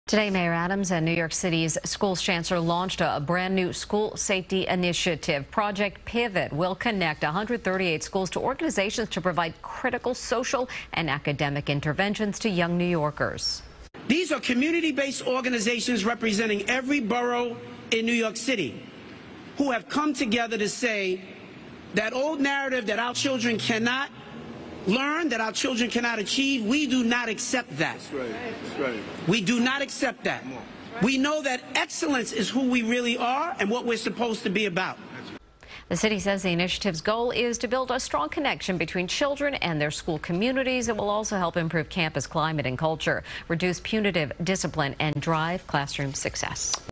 NYC launches new school safety initiative
Project Pivot will connect 138 schools to organizations that provide critical social and academic interventions.